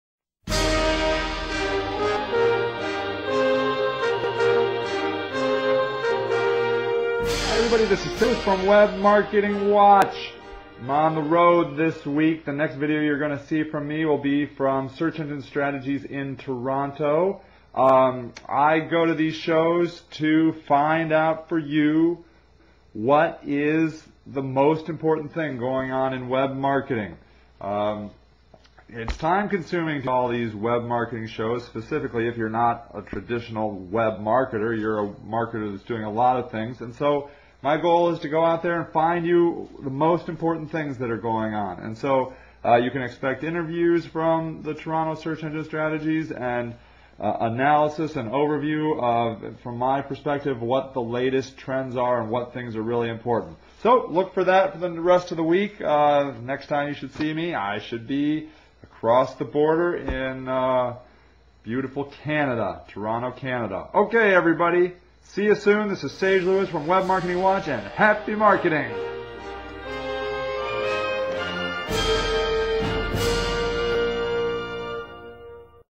Intro to Search Engine Strategies Toronto
This week I'll be in Toronto at the Search Engine Strategies Conference. I talk briefly here about why I go to these conferences and why they are valuable for you.